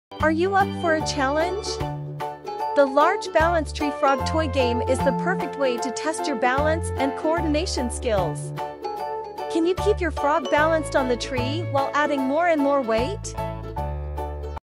Challenging Yourself with the Large Balance Tree Frog Toy Game
Are you up for a challenge? The Large Balance Tree Frog Toy is the perfect way to test your balance and coordination skills.

This toy is designed for adults and features a larger tree and frogs for added difficulty. It's the perfect way to unwind after a long day or to challenge yourself and your friends.

Can you keep your frog balanced on the tree while adding more and more weight? Give it a try and see how you stack up against the competition!